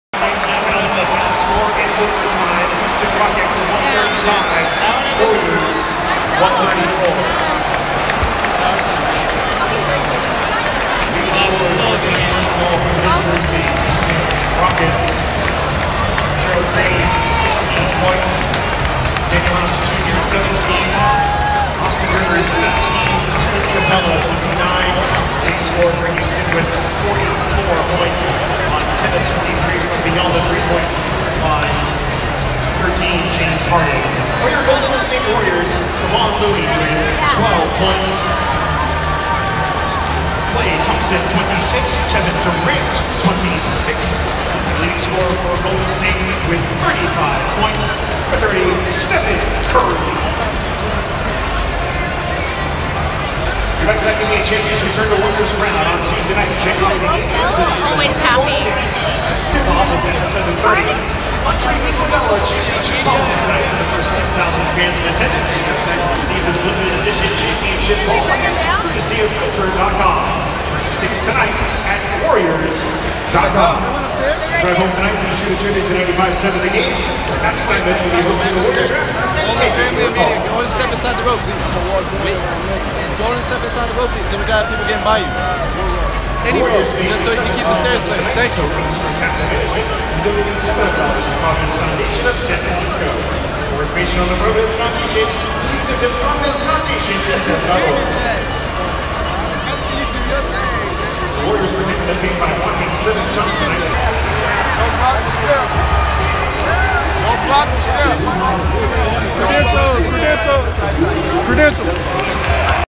Warriors (25-14) postgame tunnel incl Steph Curry, KD (Kevin Durant), Klay after loss to Rockets
From Golden State Warriors (25-14) postgame after Houston Rockets, at Oracle Arena in Oakland, CA.